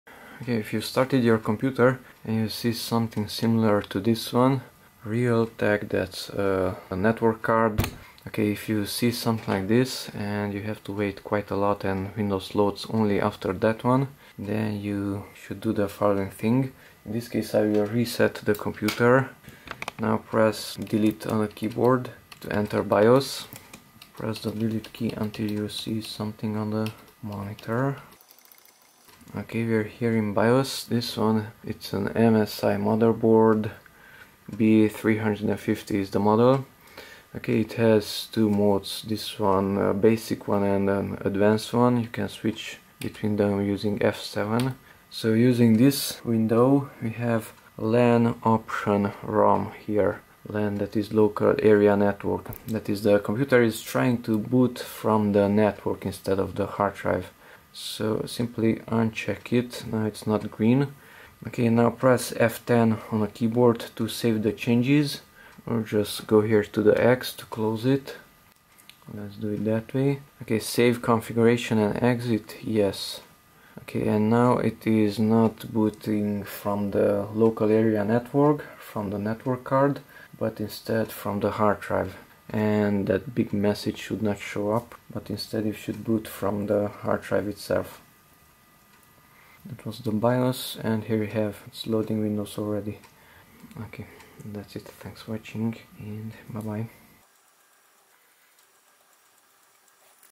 How to boot PC from Hard Drive instead of LAN (MSI B350 PC MATE)
The following messages are shown when the MSI B350 PC MATE motherboard is trying to boot from the network (LAN):
Intel UNDI, PXE
Realtek PCIe GBE Family Controller
CLIENT MAC ADDR:
DHCP...

Booting from LAN can be disabled from the Advanced Menu also:
- Settings
- Advanced
- Integrated Peripherals
- LAN Option ROM (Disabled)